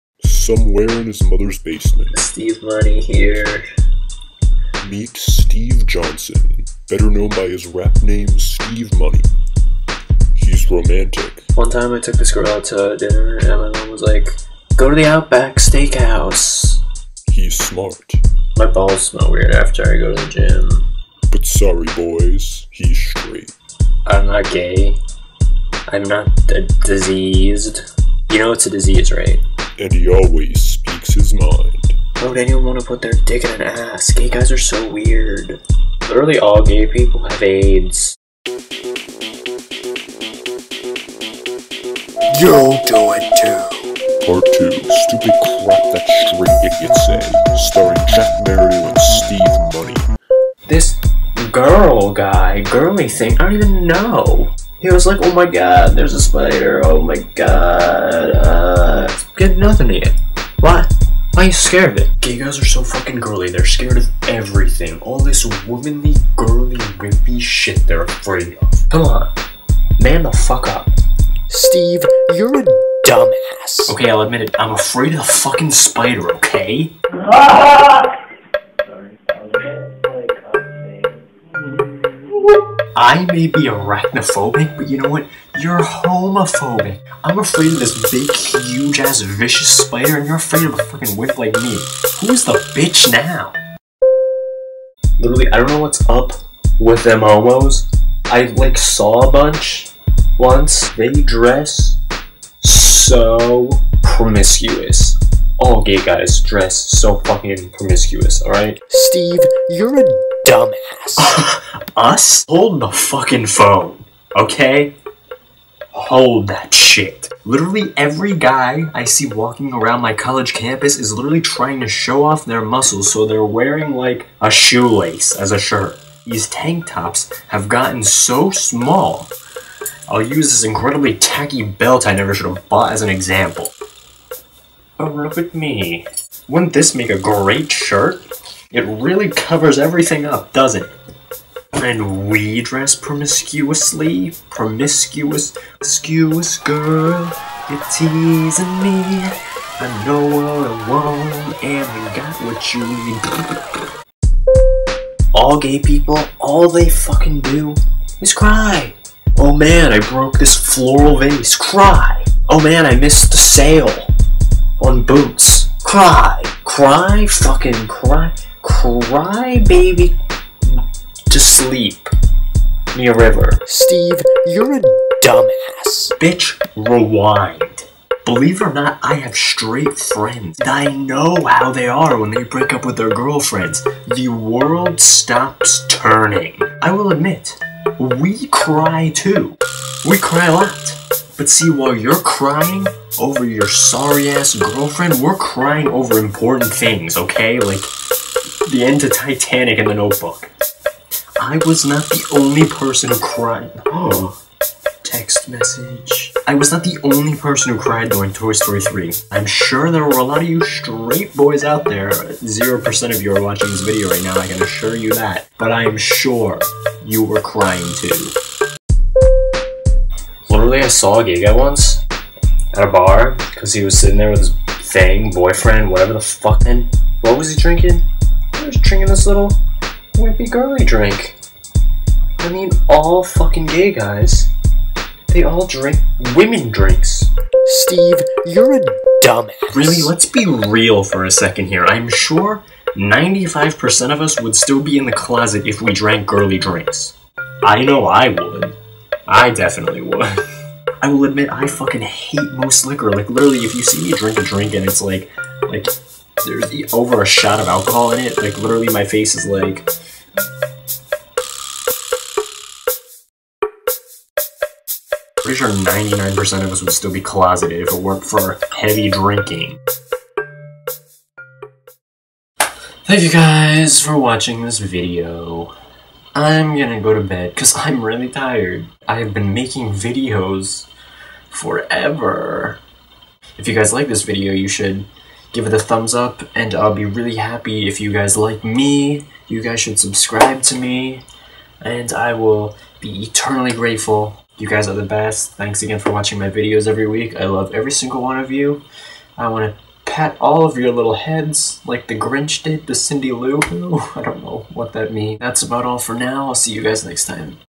YOU DO IT TOO! (Part Two)
The sequel to YOU DO IT TOO! I needed help with this video, so I called up my boy Steve Money. If you happen to be a member of the 0.01% of straight men who watch my videos, please, don't be like Steve Money. We won't like you very much.